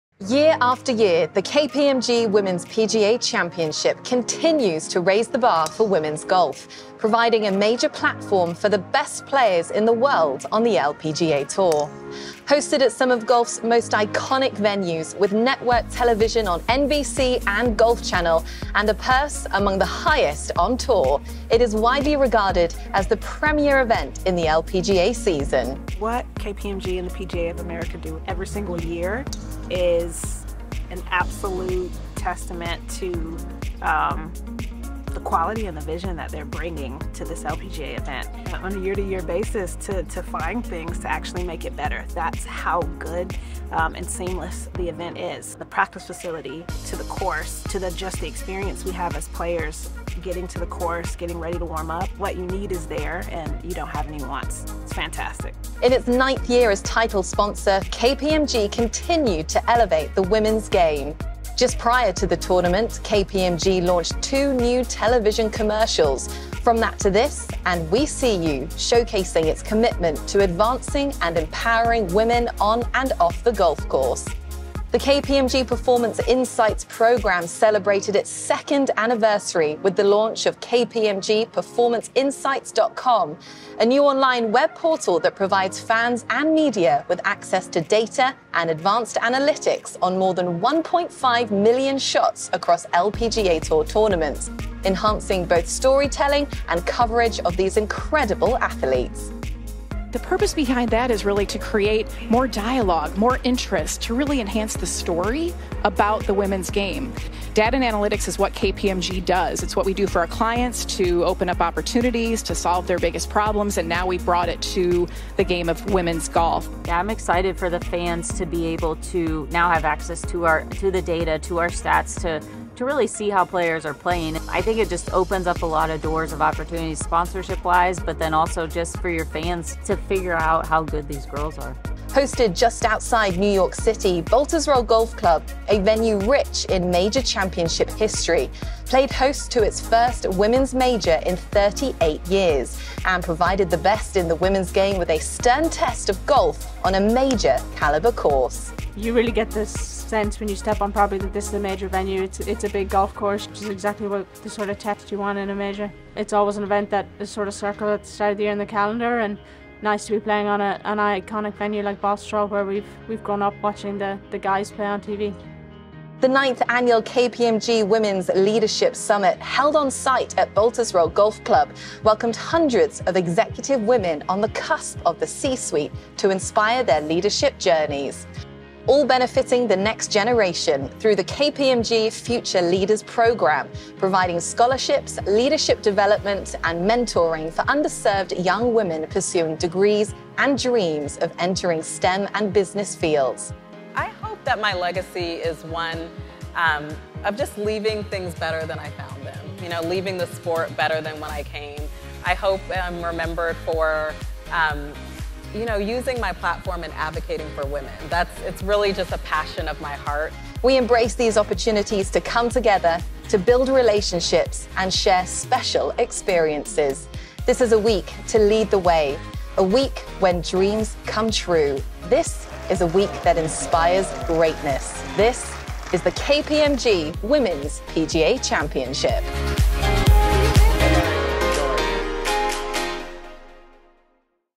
2023 KPMG Women's PGA Championship Recap
A look back on another incredible week at the 2023 KPMG Women's PGA Championship at Baltusrol.

For more on the KPMG Women's PGA Championship and KPMG Golf program, visit golf.kpmg.us.